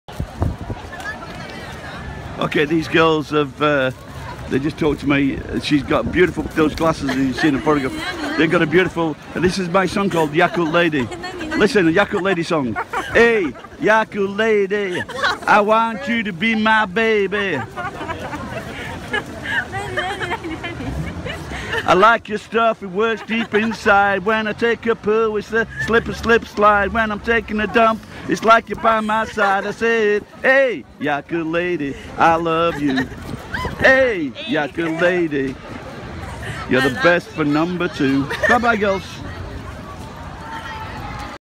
Singing My YAKULT song to japanese girls
I wrote this song years ago and on the spur (I was srunk)